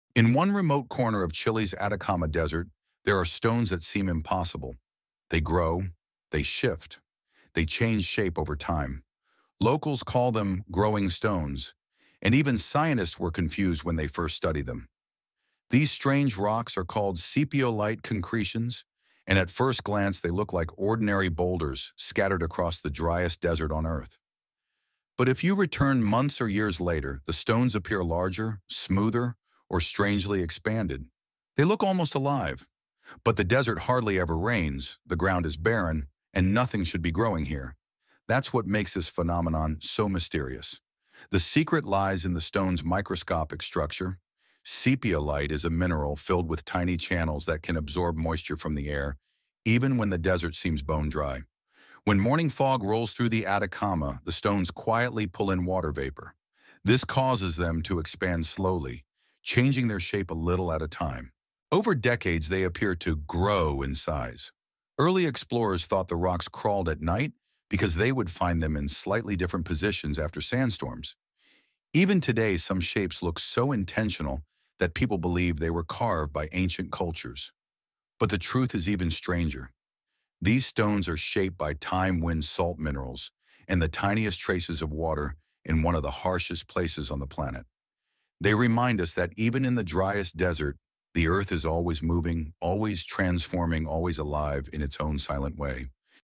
The Desert Rocks That Grow Like Living Creatures
A story about Chile’s mysterious “growing stones,” how sepiolite minerals absorb moisture and expand, and why these desert rocks seem alive.

Verified Sources

✅️ Geological Society mineral structure reports
✅️ Chilean Atacama Desert field studies
✅️ Journal of Sedimentary Research on sepiolite formations